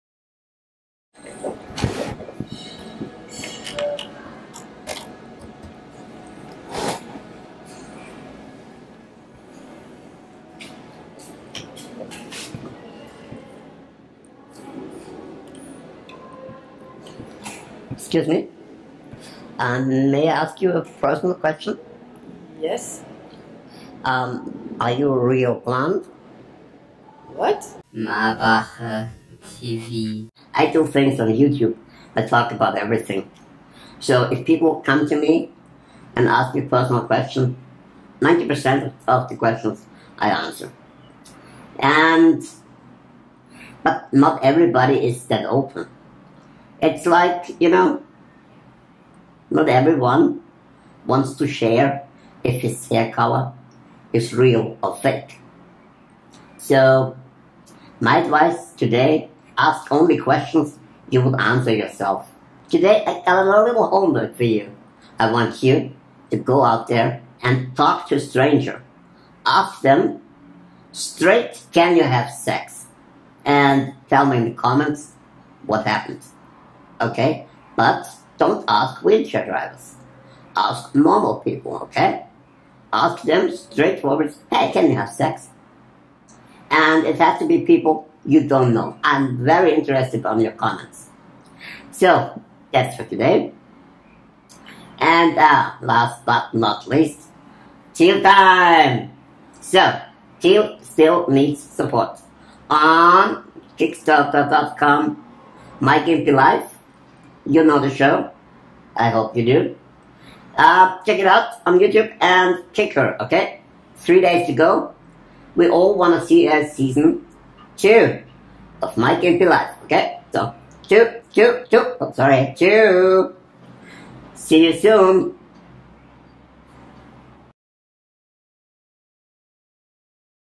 MGL - 3 Days! May i ask
- to make sure Teal is getting ready for a second season of MGL!

So kick her on Kickstarter.com — and become a Backer!

I will release every day one video with one (hopefully) usefull advice to 'How To Talk To People In Wheelchairs?' Hashtag: HTTTPIW.
At the end of each video i will reminde you to „kick" Teals defenetly perfect hidden, but with absolutely certainty cute ass on kickstarter.com! ;-)

The campaign will end on June 14th.

Looking forward to MGL season 2!!!